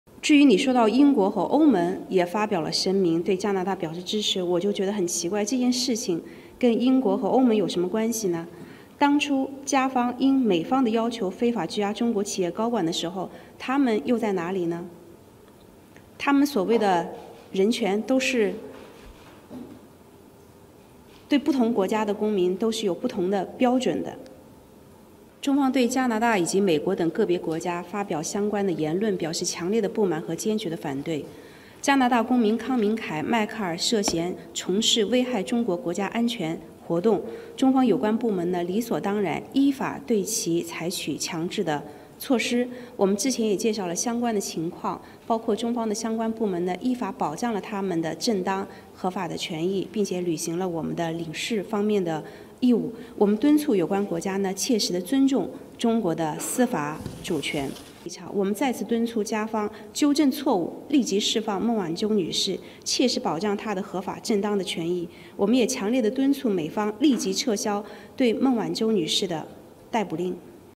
China accuses Britain, EU of hypocrisy over support for Canada's detention concerns
China accused Britain and the European Union of hypocrisy on Monday over their support of Canada's detention concerns, stating Michael Kovrig and Michael Spavor were detained for national security concerns.

The accusations come as more countries rally in support of Canada, calling on the release of the two Canadians, who China says are being held under suspicion of endangering state security.

China questioned the support, asking why they didn't question Canada over its arrest of Huawei CFO Meng Wanzhou.